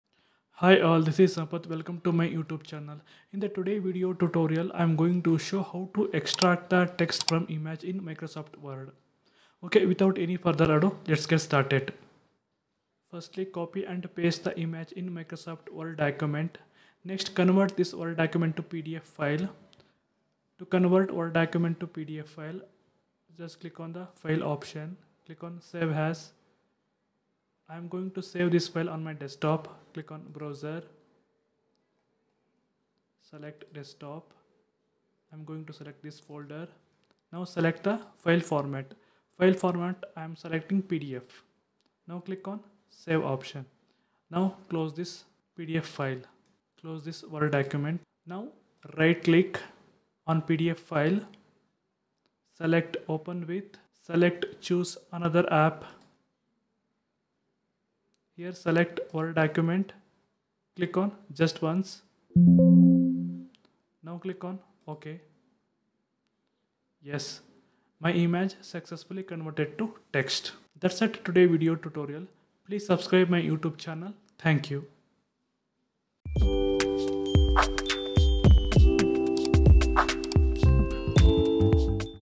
How to Extract Text from Image in Microsoft word
Struggling to copy text from a picture in Microsoft Word? You're not alone! This video will show you a quick and easy way to extract text from images.